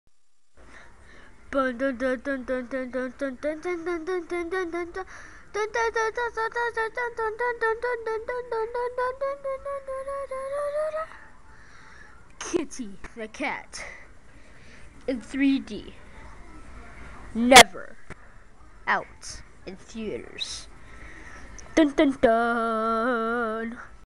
Kitty the Cat
I was bored so I took my new camera and made this video with my cat named sofi.